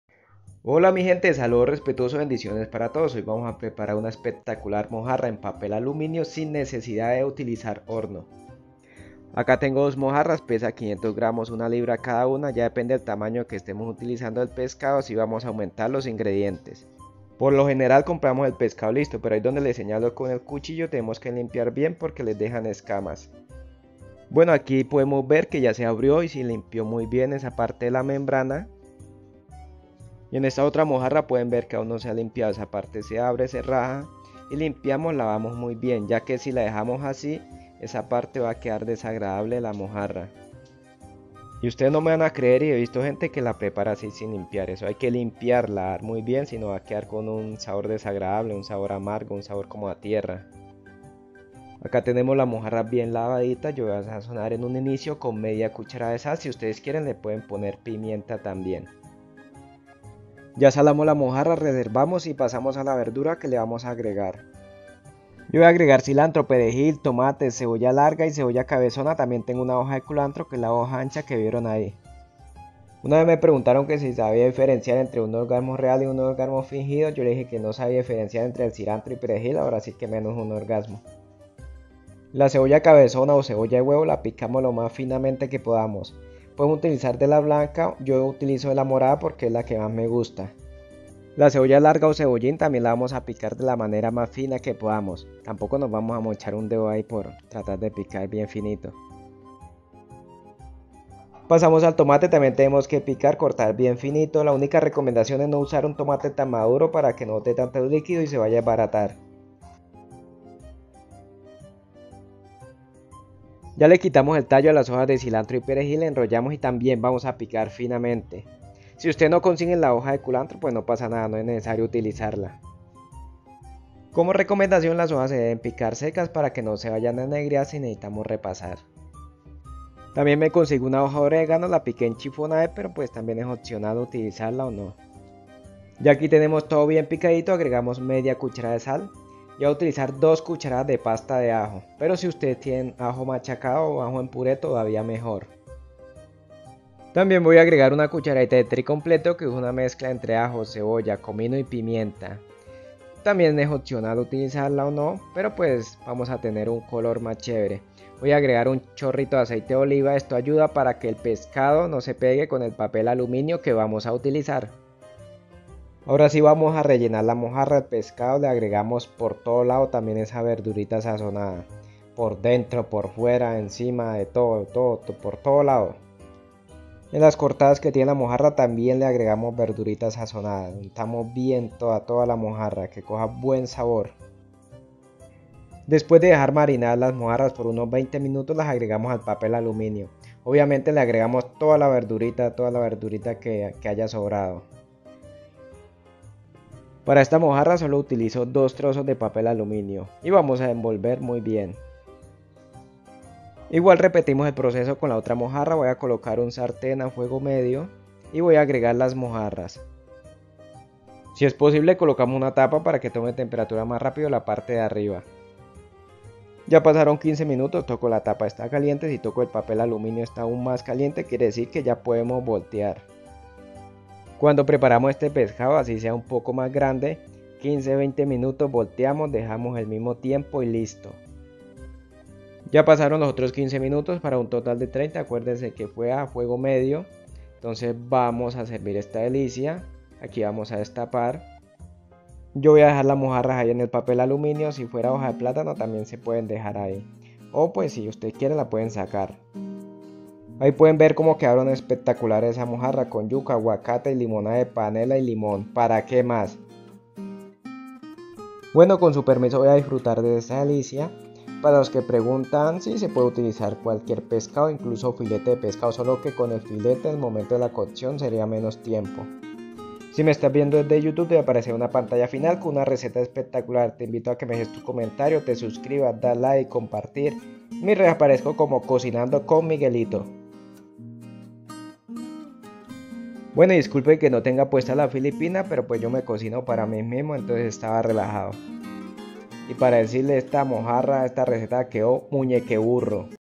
Mojarra en Papel Aluminio sin Horno - Pescado en Cabrito - Mojarra en Cabrito - Semana Santa.
Hoy tenemos Mojarra en papel aluminio sin horno, una espectacular mojarra asada en papel aluminio. Una receta súper rica y muy fácil dónde se aprovechan a máximo los sabores de la verdura y el pescado.
No necesitamos horno para preparar está delicia lo que es mejor. Receta muy similar al pescado en Cabrito.